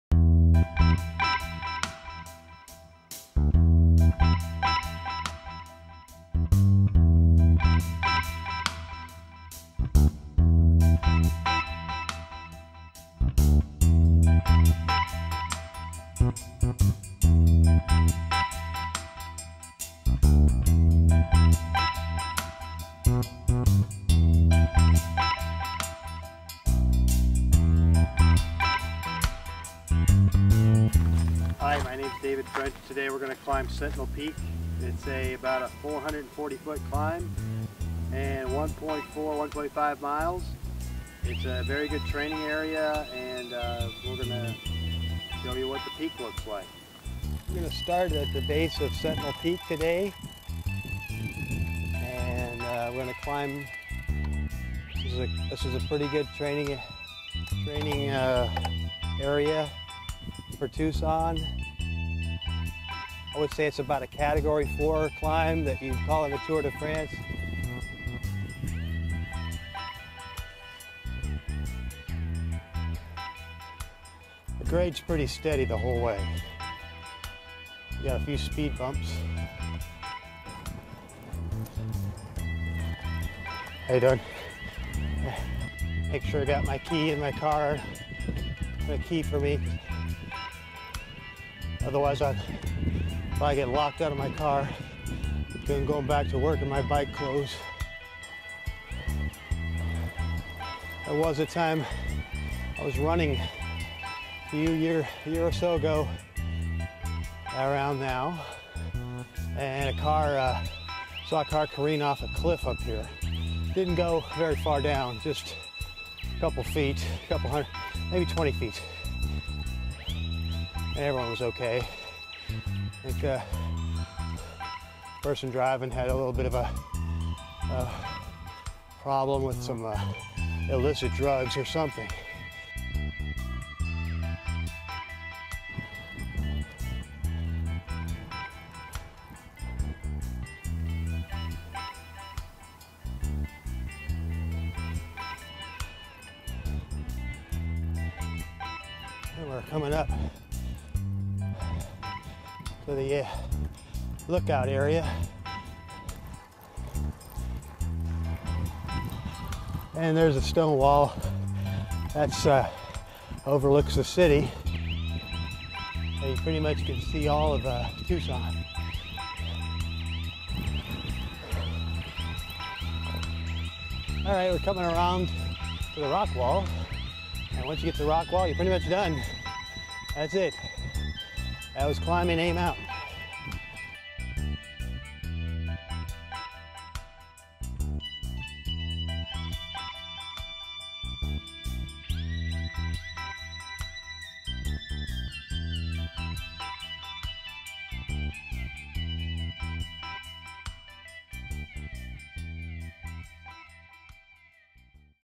Cycling up Sentinel Peak
I needed to practice some editing for an upcoming Final Cut Pro X class I will be taking in the fall and a needed a subject and a venue - so what you get is me! This is a short hill overlooking the city of Tucson, once a lookout point, it has a big "A" on it for the University of Arizona.  It is known as both "A" mountain and Sentinel Peak.  This was filmed with a GoPro Hero 2 and a JVC GY-HM150, edited with Final Cut Pro X.